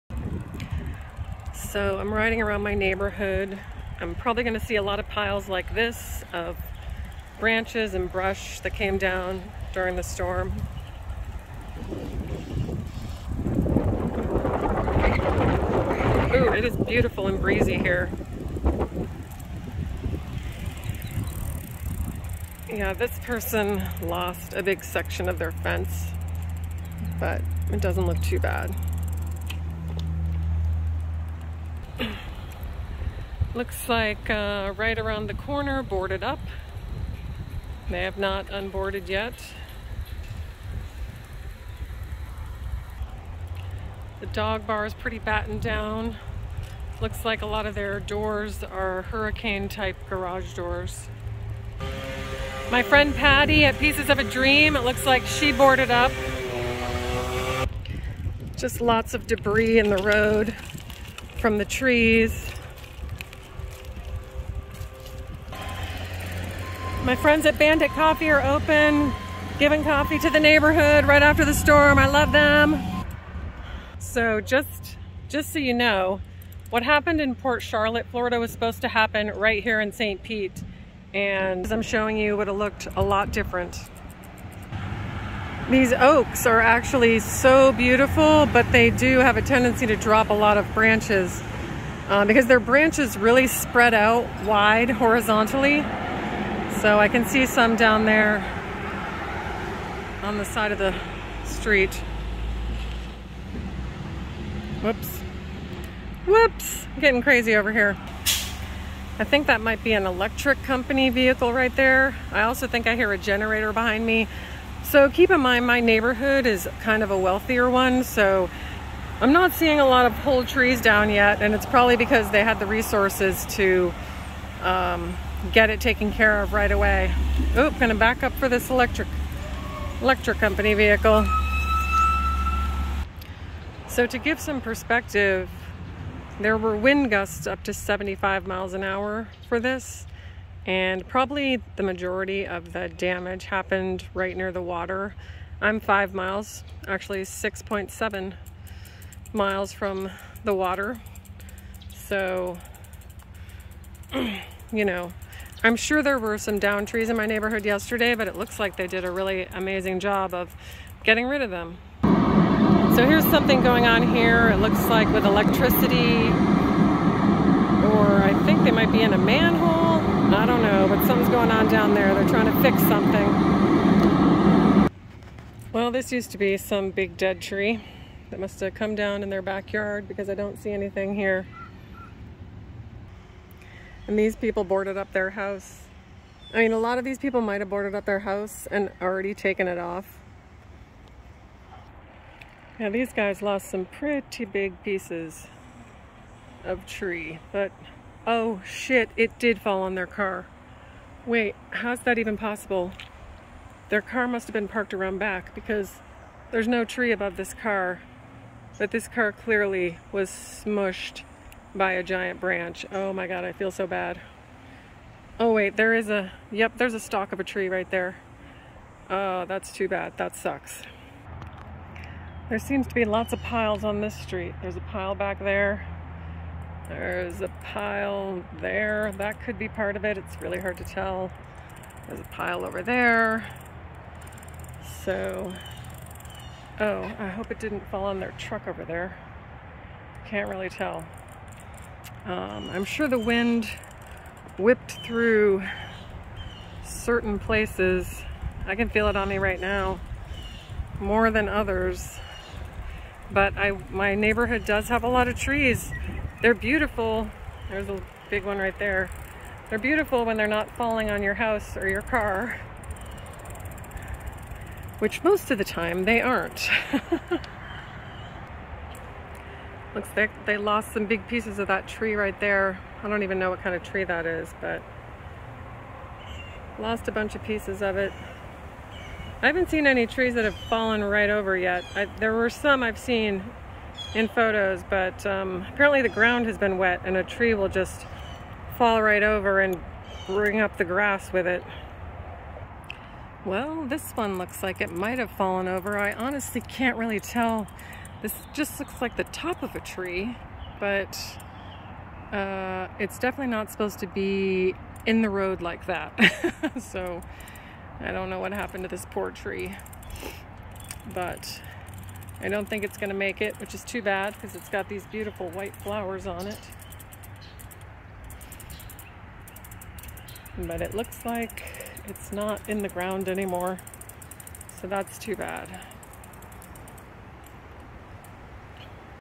Hurricane Ian in St. Petersburg, Florida - September 2022
Just a short bike ride around my neighborhood to see what Hurricane Ian did. We were supposed to be Ground Zero for this hurricane, but we got really lucky.

Give your loved ones extra hugs today. The devastation in southwest Florida is tragic and may take years to rebuild. I'm so grateful for the blessing I got and so sad for the terror and loss others experienced.